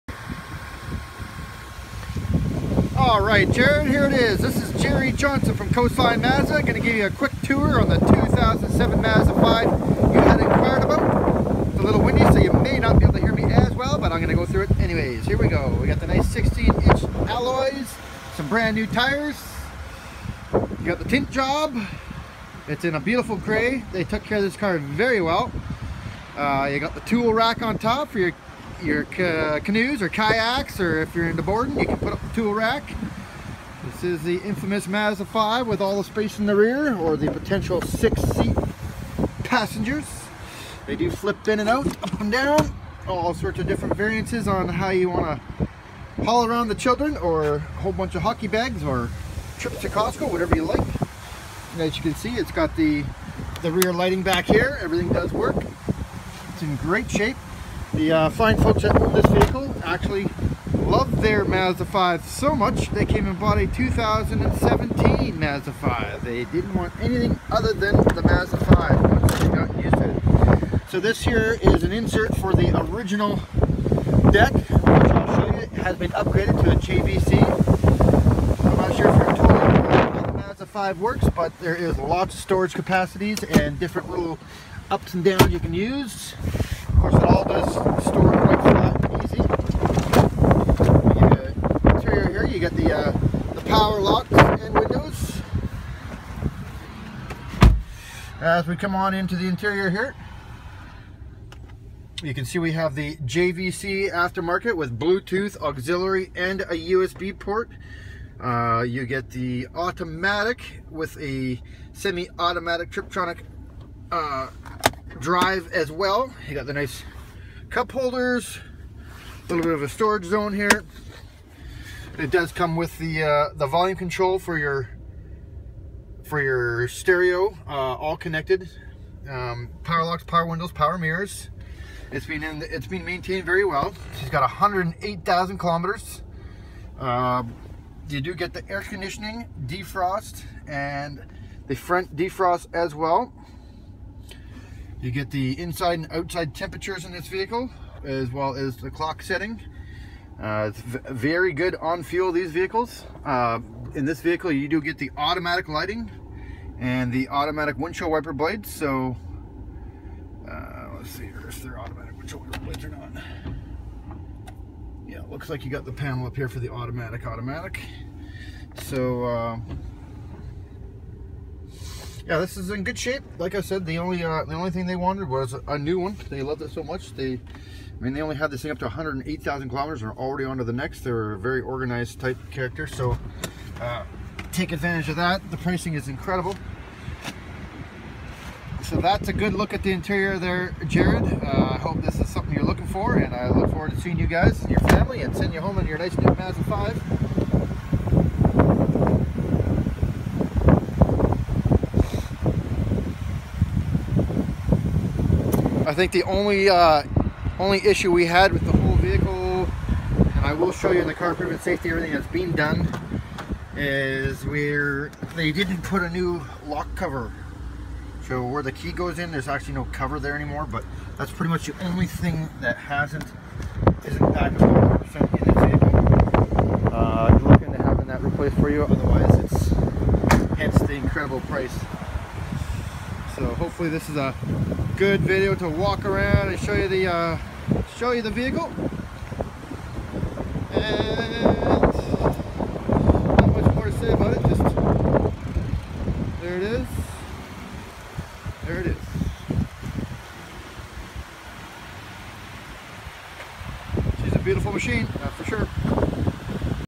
2007 Mazda 5
This is a fantastic vehicle for growing family's that are always on the go. Seating for 6 with all the essentials and a couple great extra's.